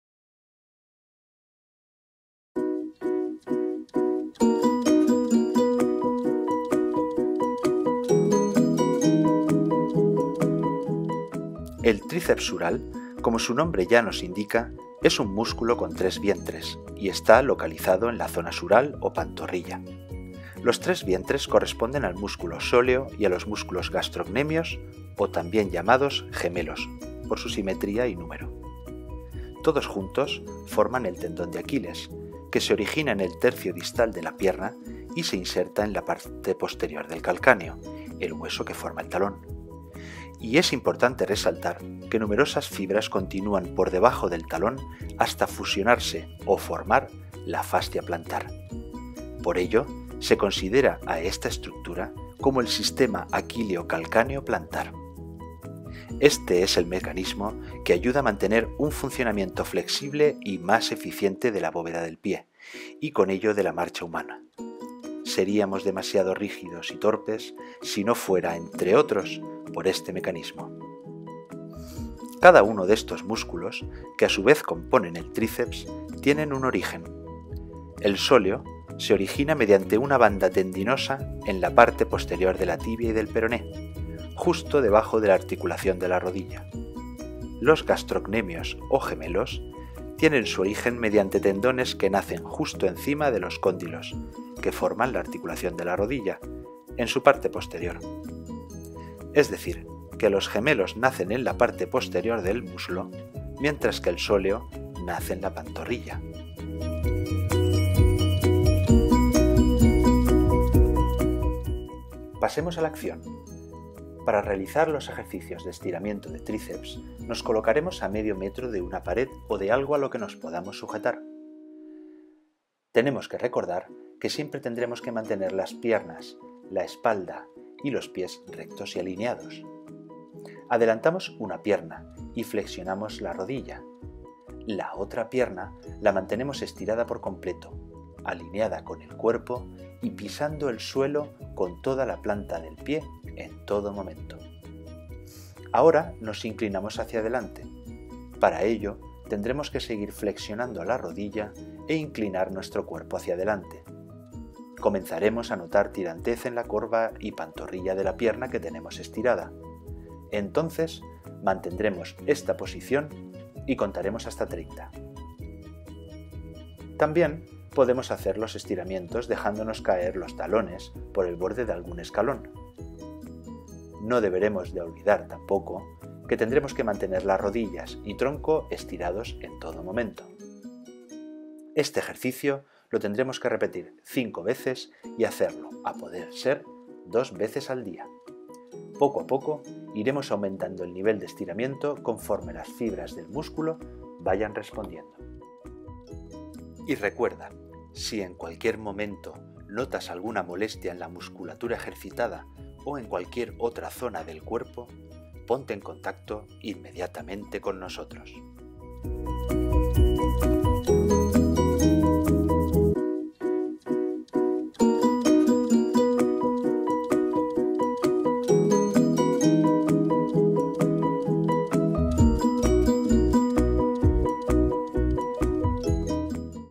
TRICEPS SURAL: ejercicios de estiramiento
Si en 100 Pasos te hemos prescrito estiramientos de Triceps Sural, observa este video-tutorial con atención. En él te enseñamos cómo se hacen los ejercicios y las pautas que debes seguir. Si tienes alguna limitación que te impida hacer los que mostramos en el video, ponte en contacto con nosotros que adaptaremos esos ejercicios a tu condición. y @100pasos